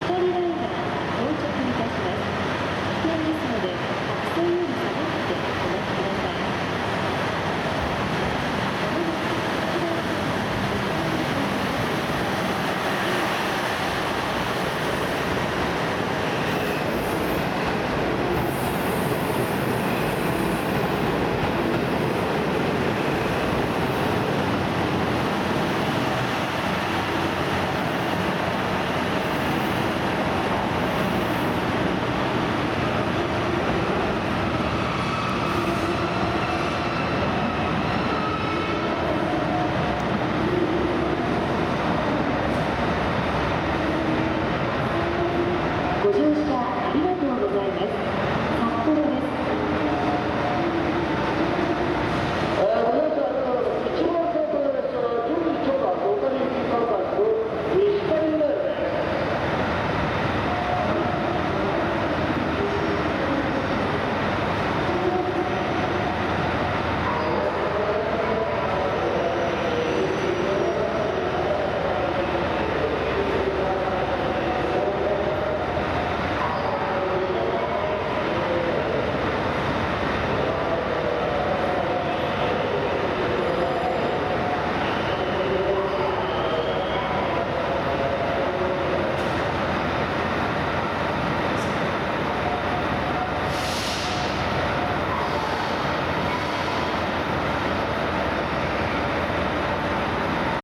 【JR北海道】特急オホーツク（キハ183系）～回送
JR北海道の特急オホーツク（キハ183系）が回送して札幌駅西側の引き込み線に待機します。